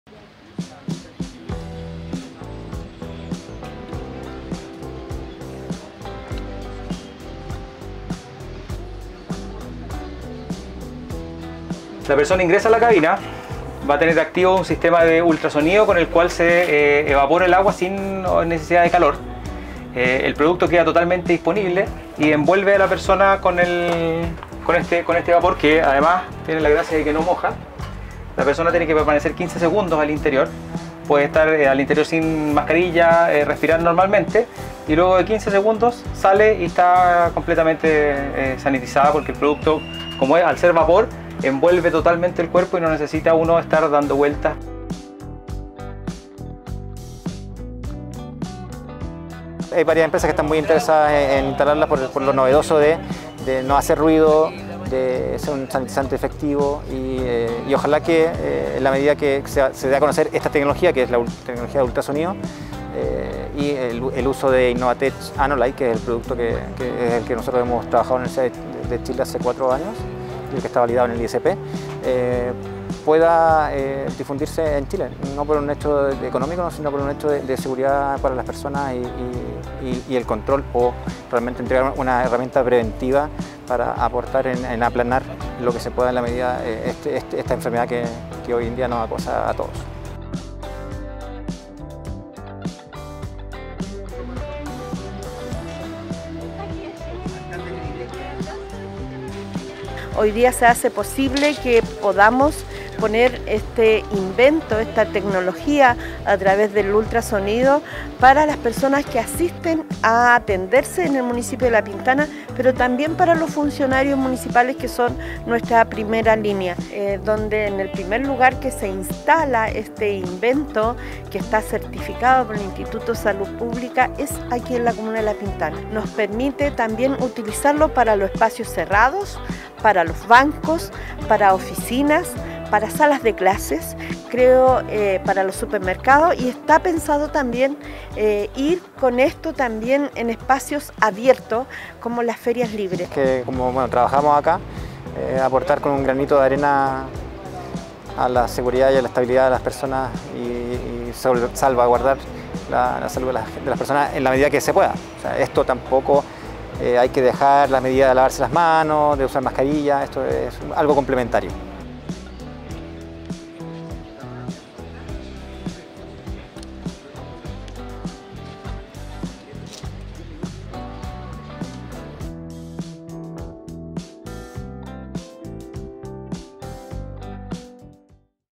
DONAN CABINA SANITIZADORA ULTRASÓNICA
Es la primera Cabina Sanitizadora ultrasónica inocua para el ser humano. No daña la piel ni el medio ambiente. Creada por investigador de la Facultad de Ciencias Agronómicas de la Universidad de Chile, que junto a la empresa FoodTec, la pusieron a disposición de La Municipalidad de La Pintana. La cabina cuenta con la aprobación del Instituto de Salud Pública. Un nuevo aporte para el combate del coronavirus